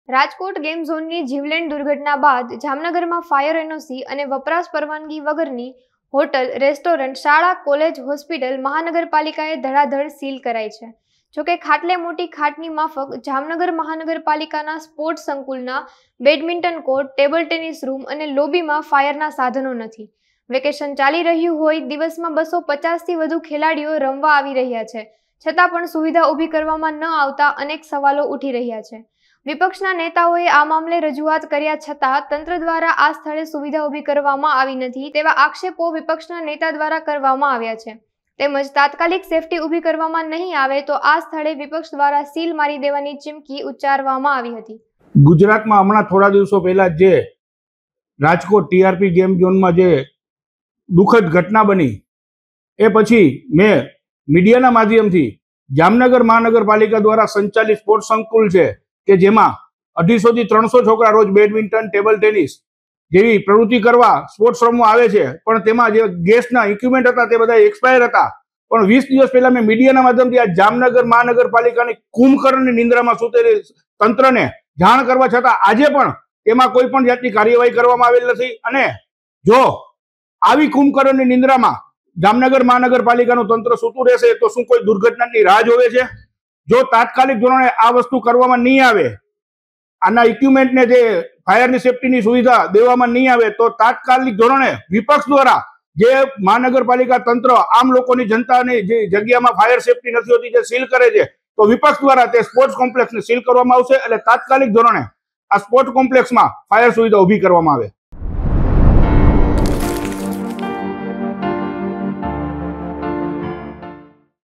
Jamnagar : મહાનગરપાલિકાના સ્પોર્ટસ સંકુલમાં ફાયરના સાધનોનો અભાવ
જામનગર : મહાનગરપાલિકાના સ્પોર્ટસ સંકુલમાં ફાયરના સાધનોનો અભાવ